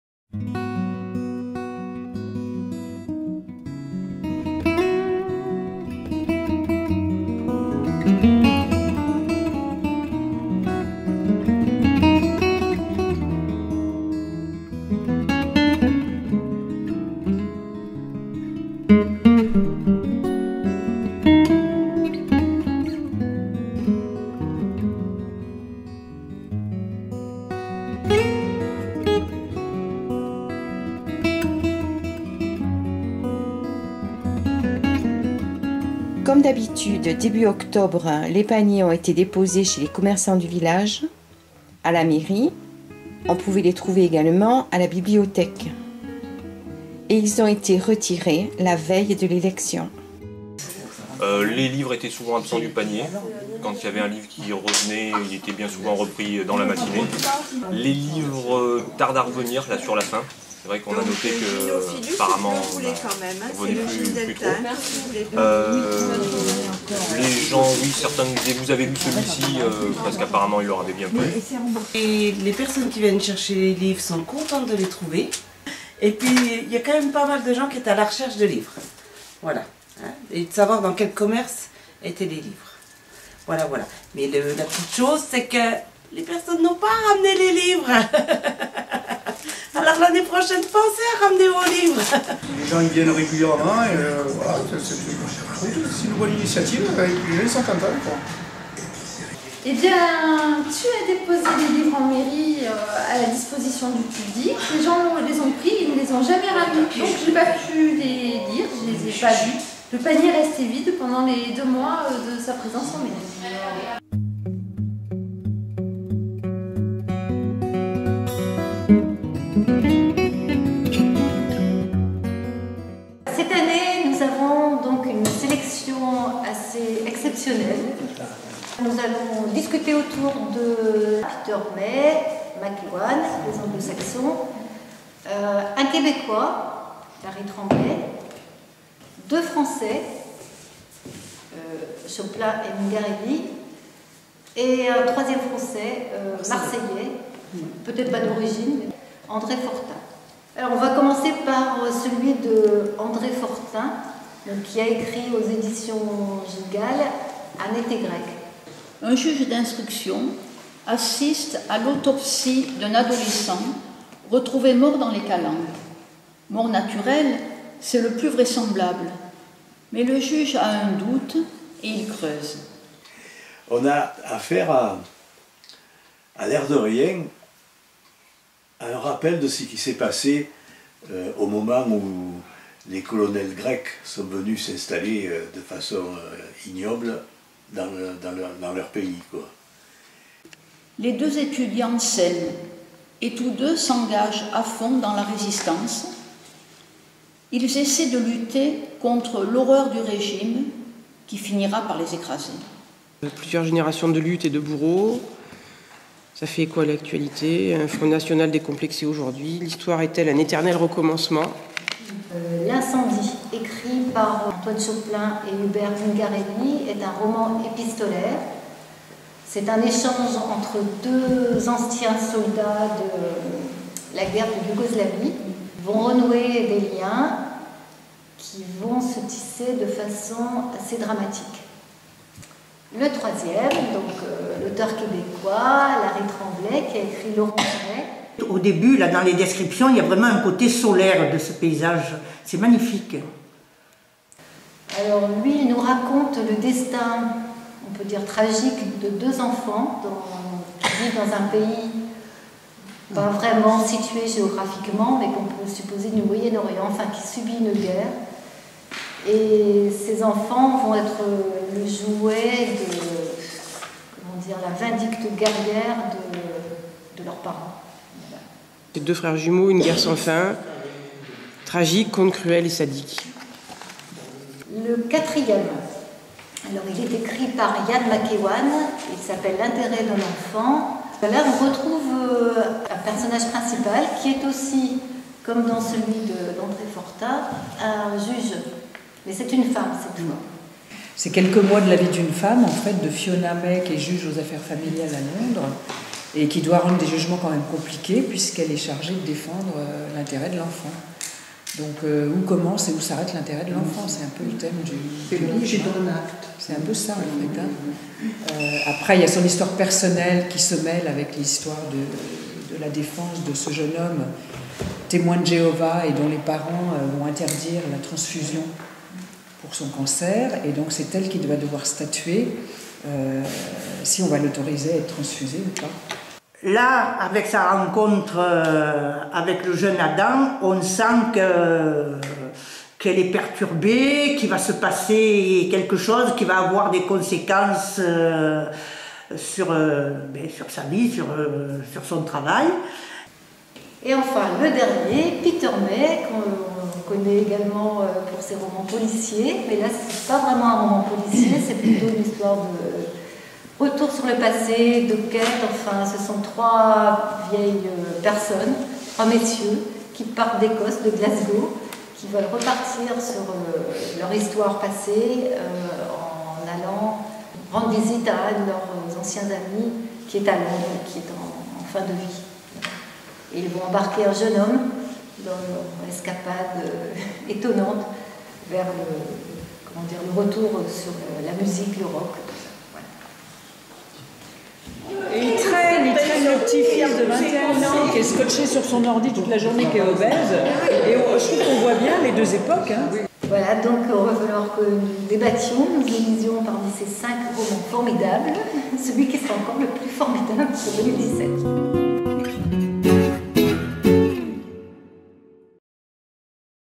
Le livre du village 2016
Présentation de la sélection du livre du village 2016.
Claude Achard.